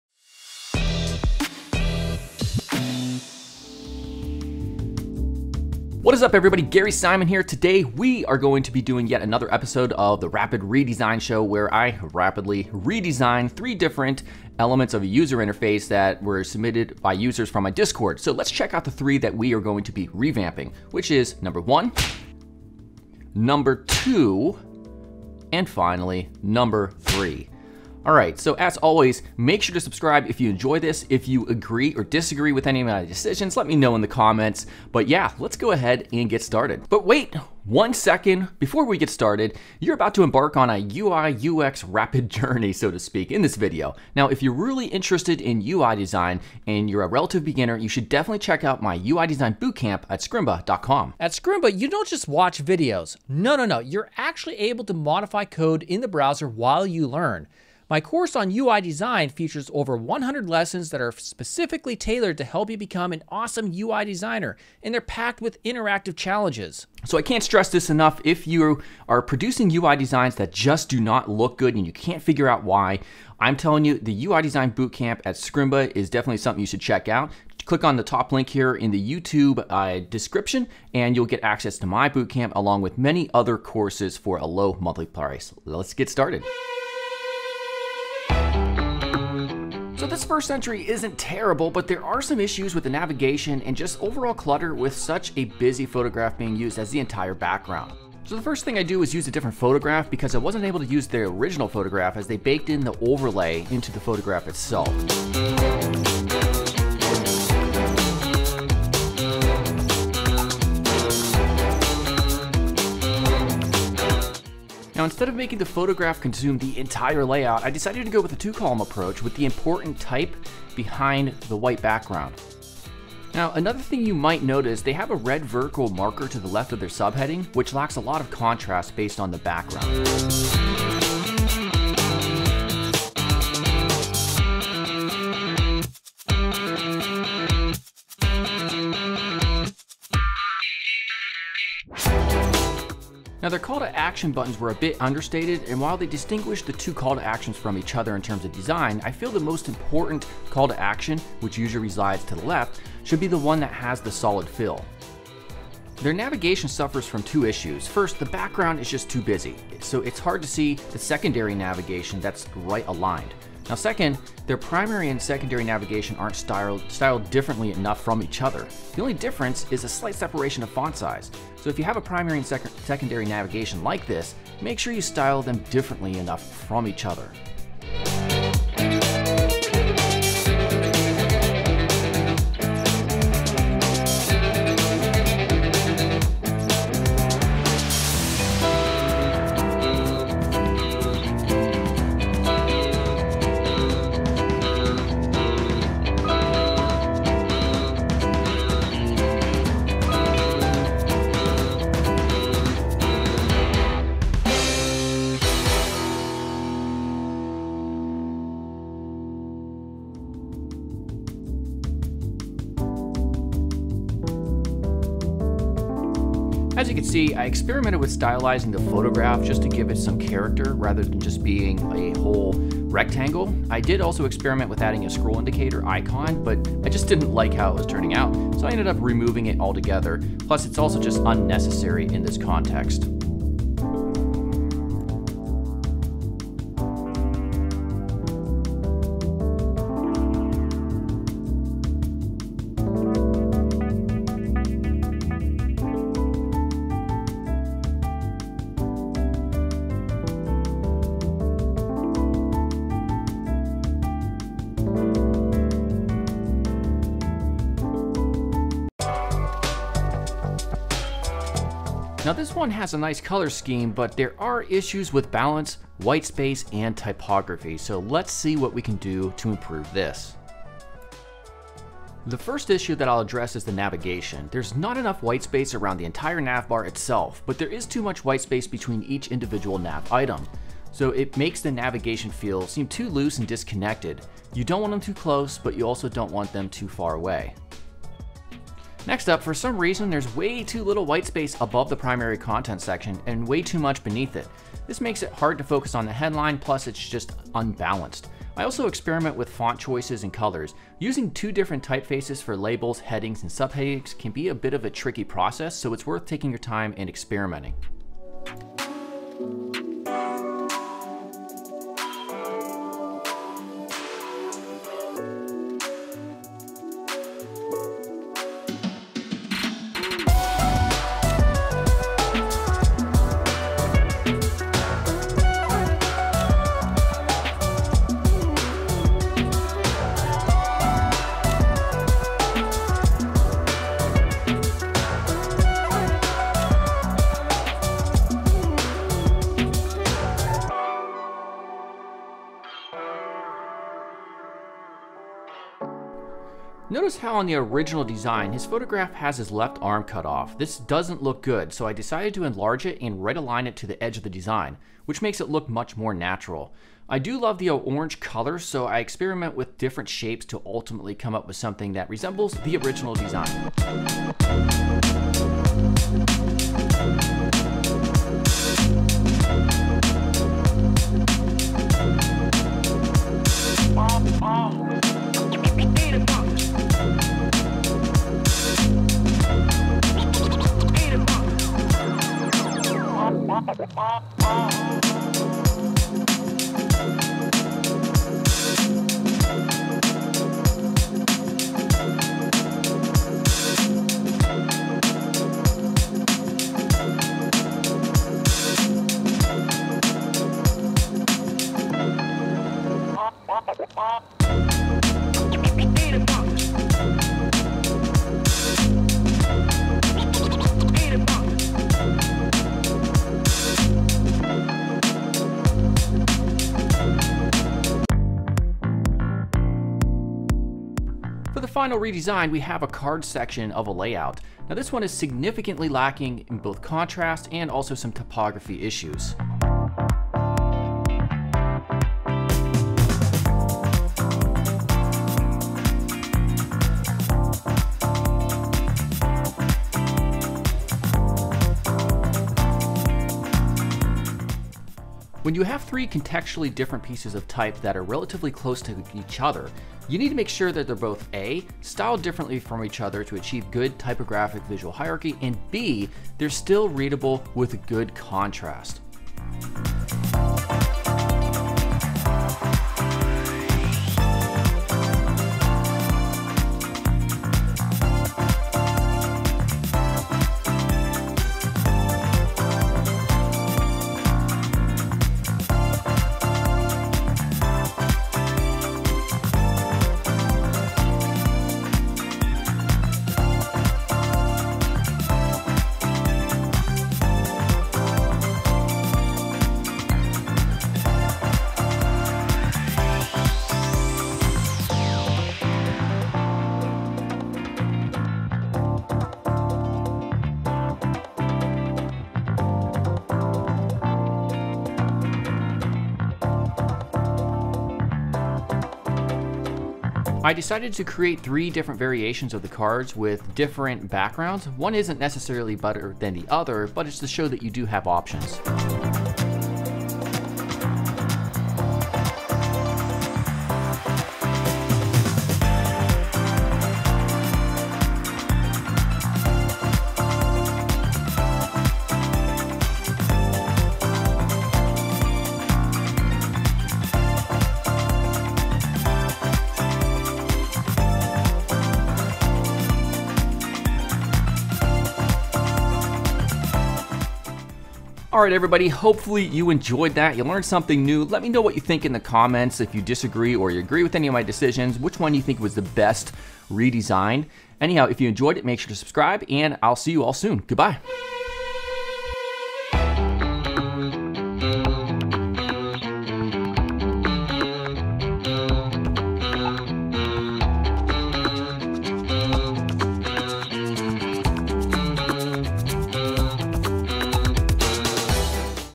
Rapid ReDesigns - BALANCE in UI Design
- Take my UI Design Bootcamp Now!
-- Welcome to the 2nd official episode of (RRD) Rapid ReDesigns! This time, we're featuring 2 hero sections and a cards section. I have to say, I think the best redesign I've done yet might be just one of these 3. Let me know which you like the most!

0:00 - Introduction
0:36 - An Awesome Offer
01:26 - Entry 1
04:19 - Entry 2
06:58 - Entry 3
09:08 - Closing